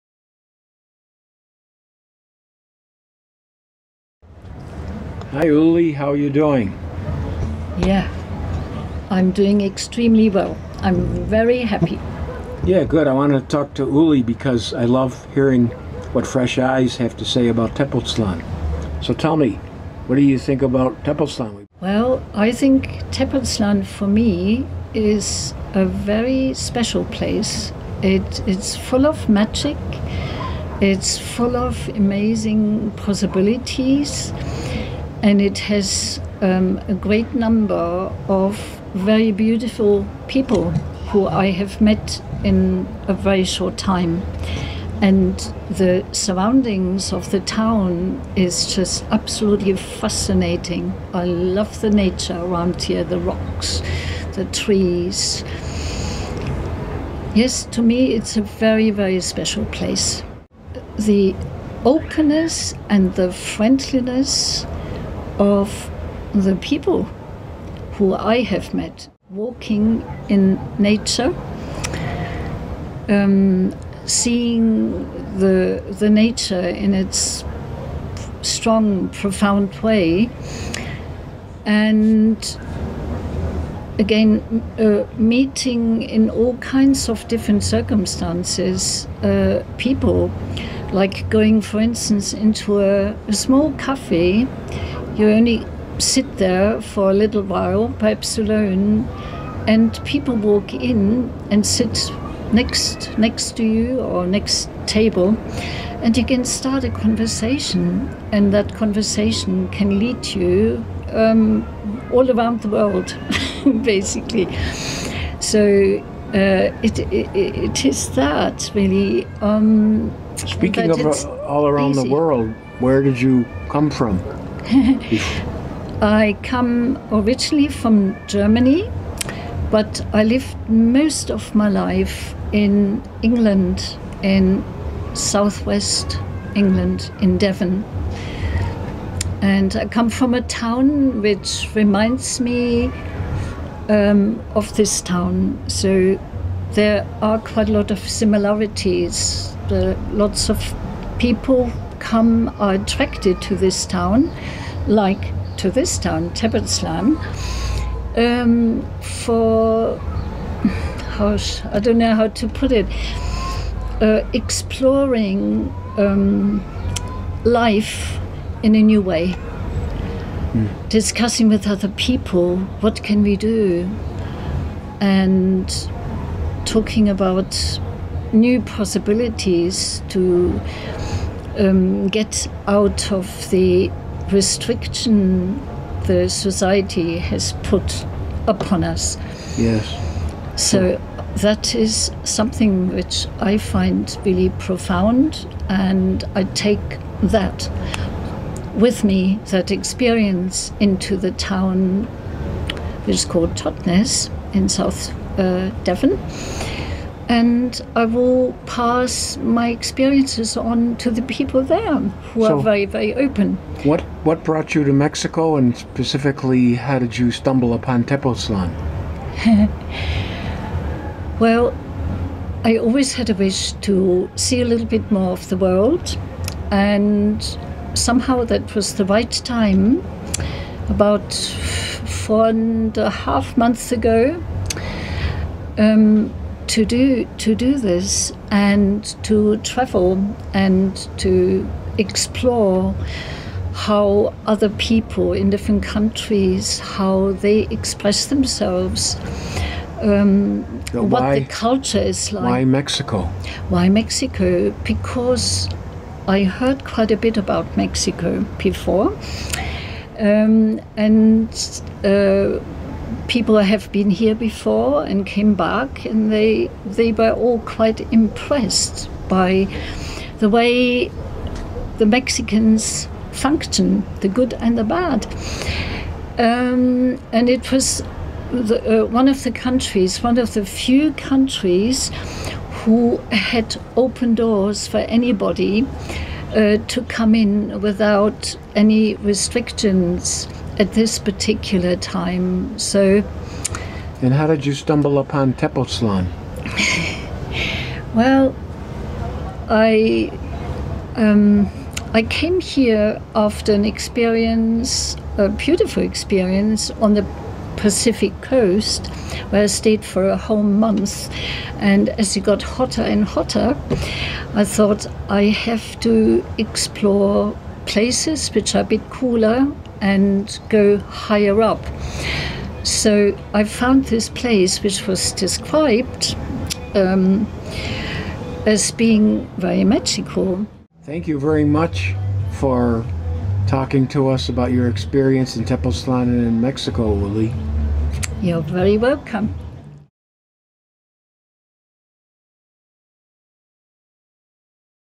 Interview with Ulli
Tepoztlán, Morelos, Mexico. I love to get the perspective of newcomers to the magic city, Tepoztlán!